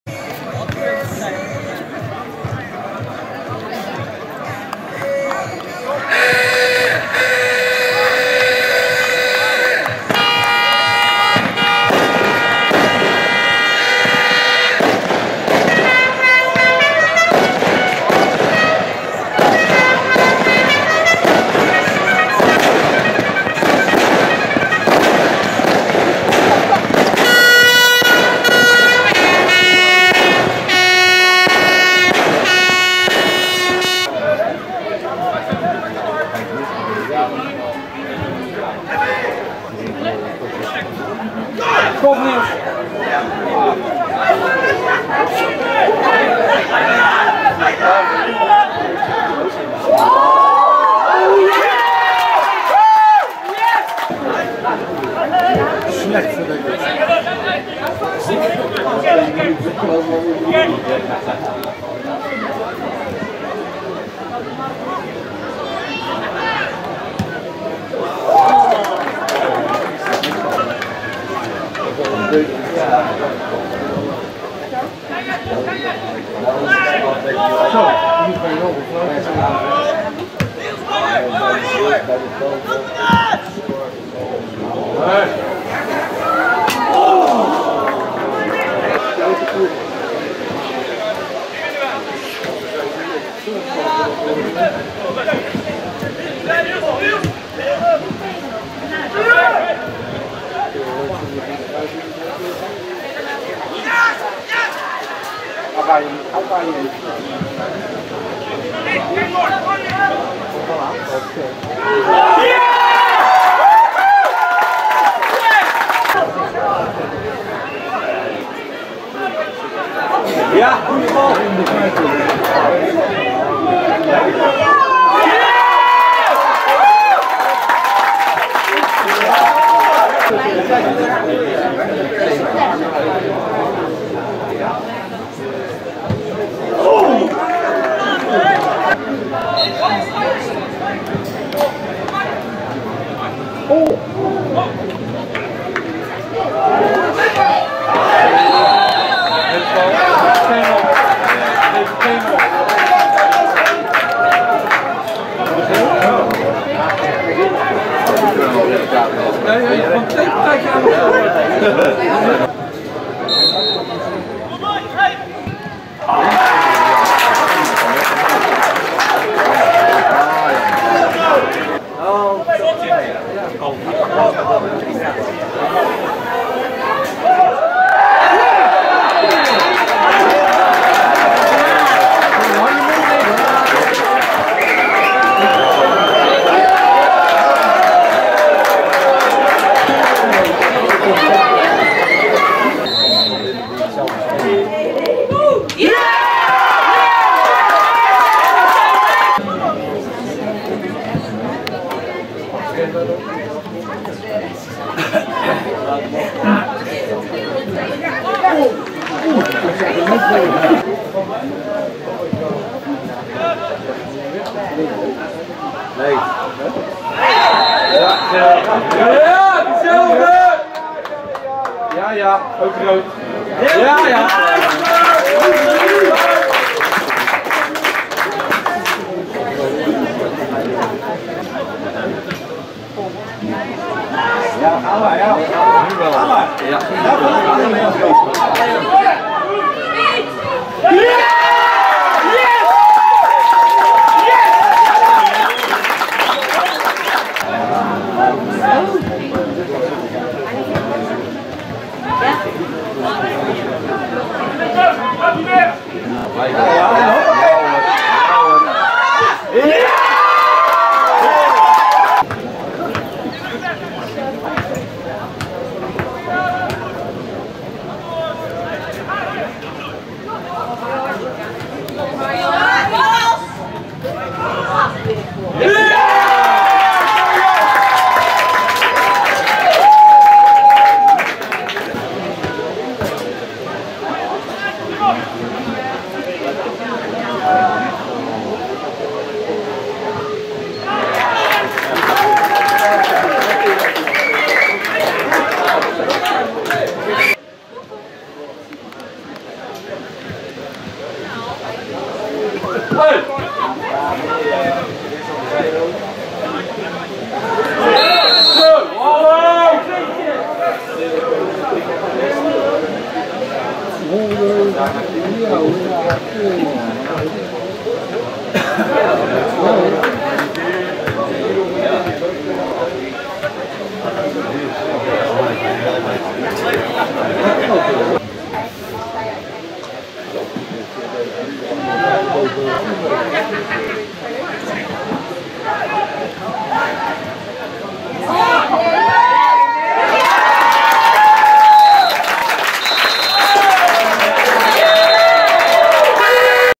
Neptunus-Schiebroek 2 - V.V. Brielle 2 (3 juni 2017)
Op zaterdag 3 juni 2017 was de promotiewedstrijd voor tweede elftallen naar de hoofdklasse tussen Neptunus-Schiebroek en V.V. Brielle aan de Hazelaarweg te Rotterdam. Het is een verkorte samenvatting geworden vanwege de ergerlijk luide scheepstoeter van iemand uit Brielle. Omdat dit geluid erg vervelend is bij het afspelen van de samenvatting ditmaal niet de gebruikelijke herhalingen van de doelpunten maar een versoberde versie.